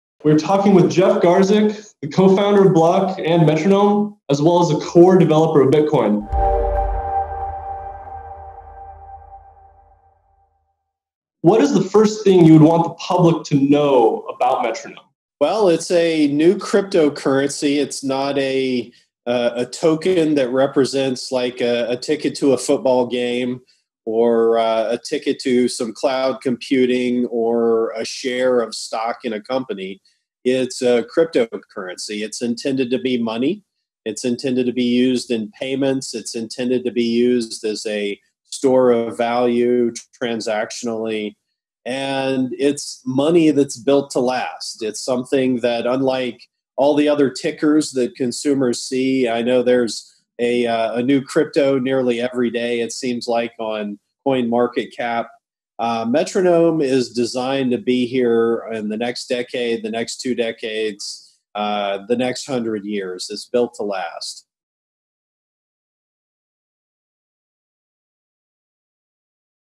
What is Metronome Cryptocurrency? - CEO Jeff Garzik Responds
We recently spoke with Jeff Garzik, an early Bitcoin developer and the CEO and Co-Founder of Bloq about their latest project, a cryptocurrency called Metronome.

Strategic Coin Crypto Market Research And Token Launch Advisory Services.

Guest: Jeff Garzik
Director/Editor: Cy Alpheus Tyrrell Gowdie
Host: Alexander Dyson